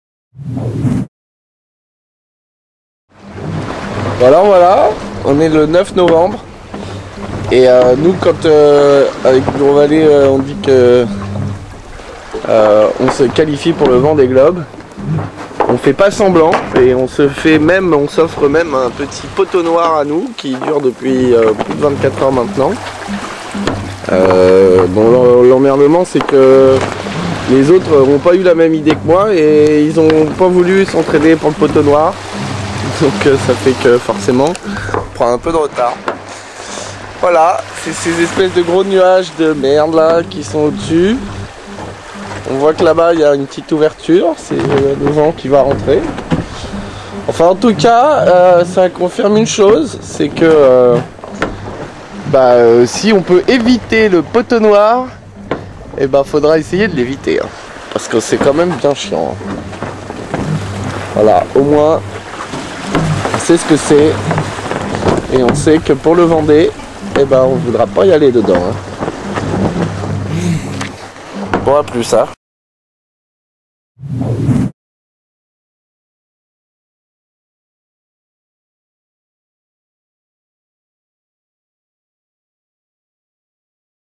Louis Burton englué à bord Bureau Vallée sur la Transat B to B
Louis Burton est englué dans une zone sans vent, il est dans won pot au noir. Au dernier classement Louis et son IMOCA Bureau Vallée étaient 8ème au classement de la BtoB 2011.